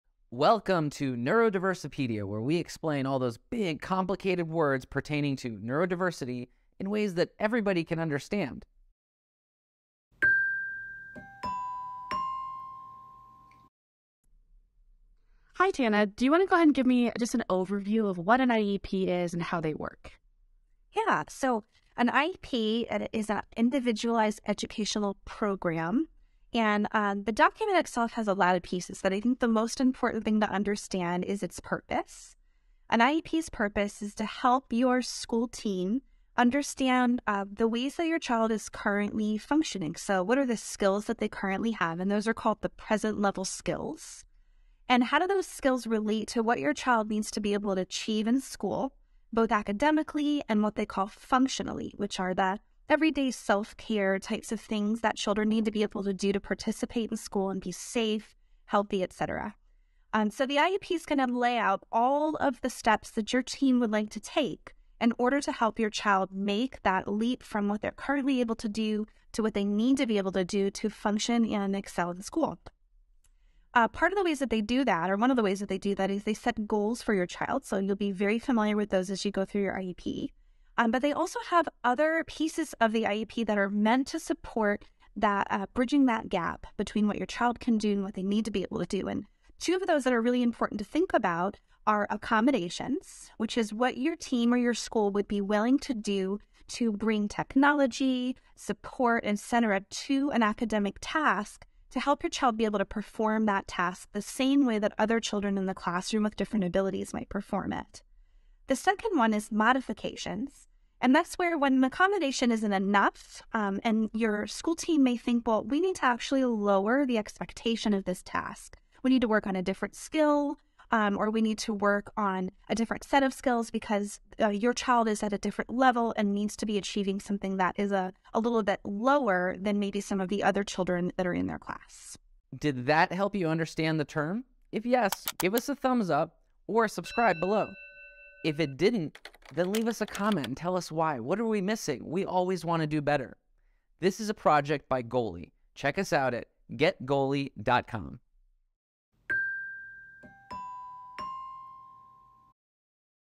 Explaining Individualized Education Programs (IEP's Made Easy)
🌟 Understanding IEPs: What Every Parent Needs to Know! 🌟

📚 In this video, we demystify the term "IEP" and provide you with the essential information you need as a parent of a neurodivergent child, such as autism or ADHD. Let's break it down in simple terms!

Timestamps:
0:00 What is an IEP?
01:00 How to use an IEP
02:00 FAQ's

🔍 What is an IEP? 🎓
An IEP stands for Individualized Education Program. It's a personalized plan designed to support your child's unique learning needs and ensure they receive the right educational assistance. 🏫✨

🔎🗒️ What does this video cover? 🎥
We answer the most frequently asked questions about IEPs: How do I request an IEP? Who is involved in the process? What goals and services can be included? We've got you covered with clear and concise explanations! 🤔💡

🔗📚 Want to learn more? Check out Goally's Neurodiversopedia page for a detailed article on IEPs and other related topics.

Thank you for being a part of our Neurodiversopedia community! 🤗✨